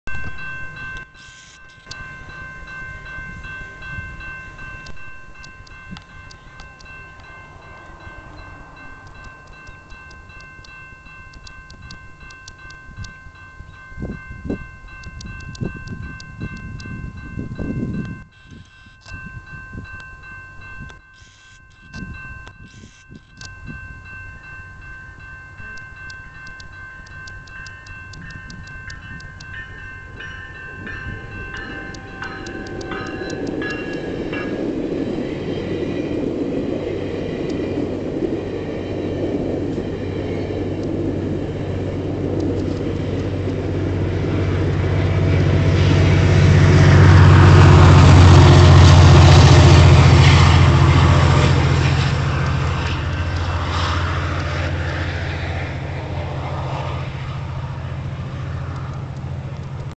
Fast Go Train At Level Crossing Backwards With Special Middle Car
Go Train going backwards, At level crossing. Loud Bell.

Nice comments and ratings please.

Filmed in Toronto Ontario Canada